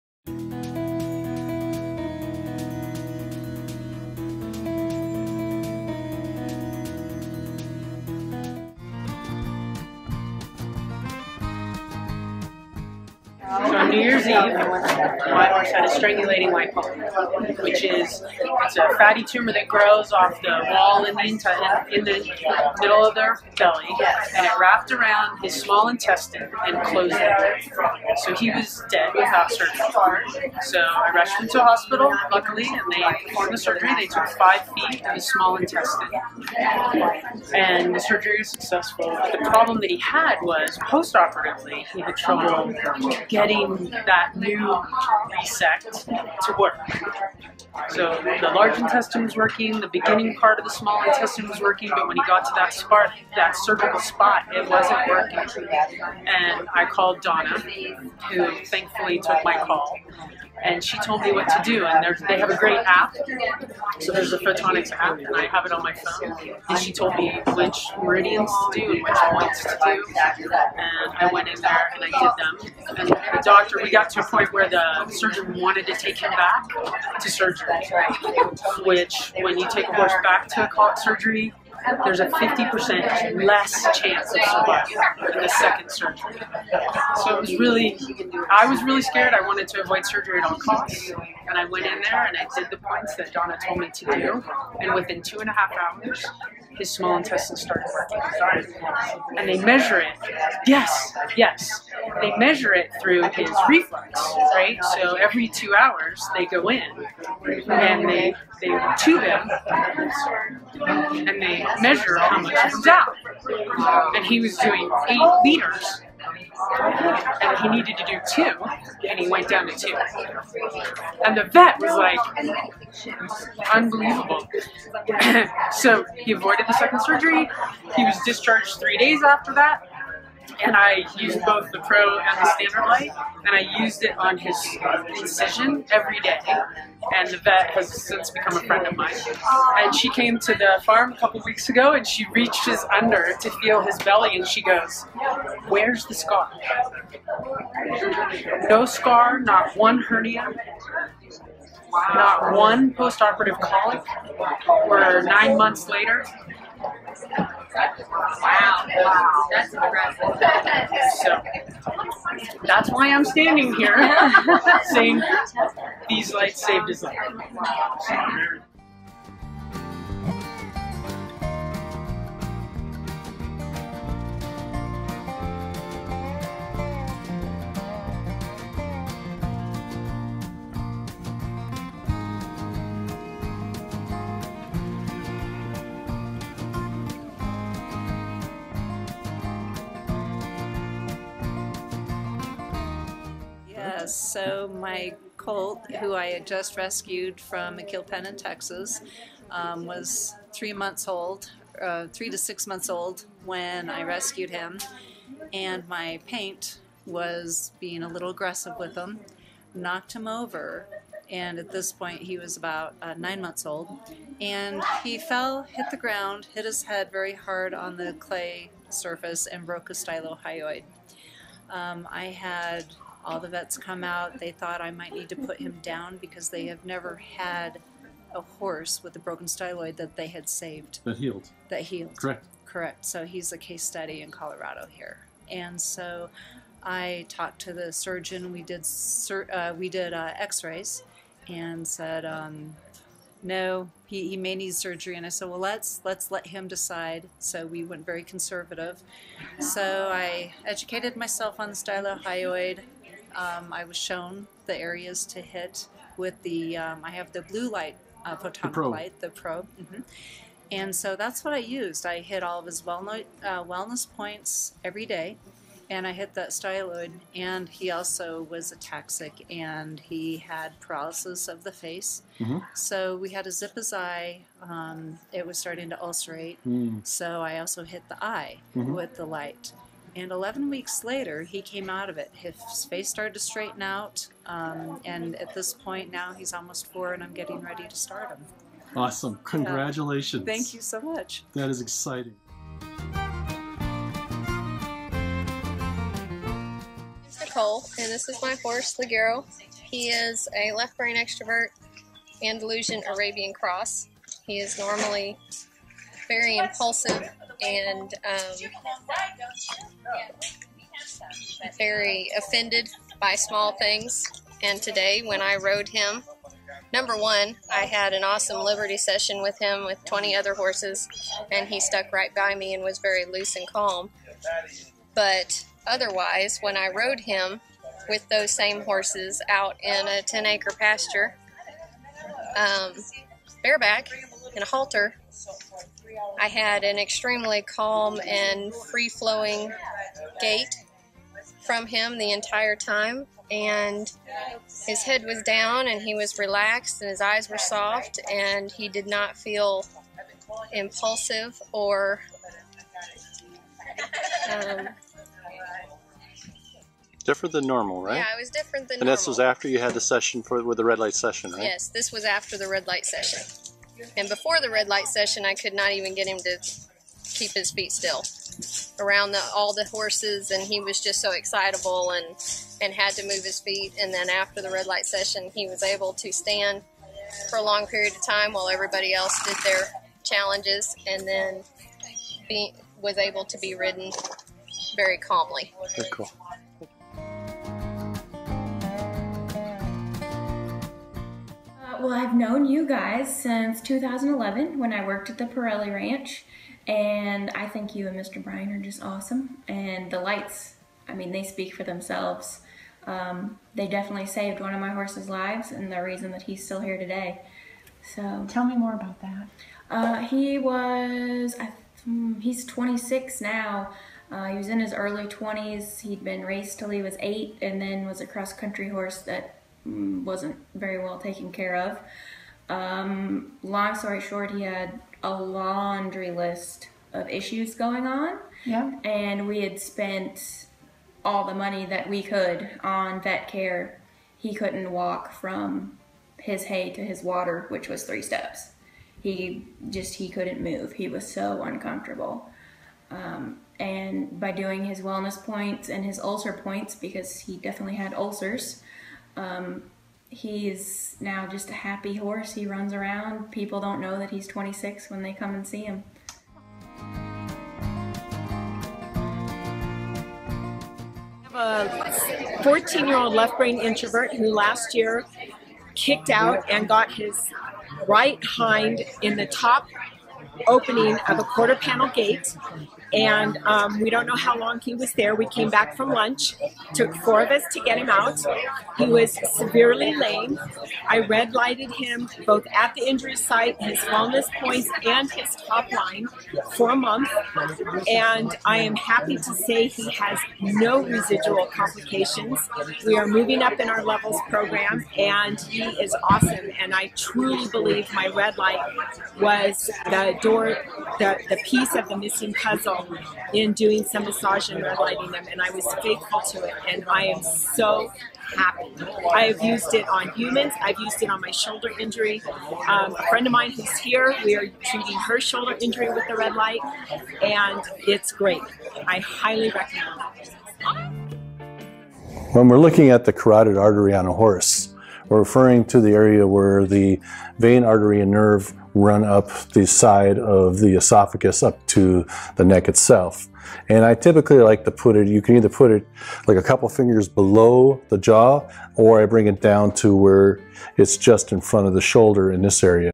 Light Therapy Success Stories: Horses on the Road to Recovery
Join us as we share the remarkable stories of horses healed by Photonic Health Light Therapy. From post-surgery complications to severe injuries, these testimonials highlight the transformative power of light. Witness the joy and relief experienced by horse owners and their equine companions

Do you have a Photonic Health Light Therapy success story?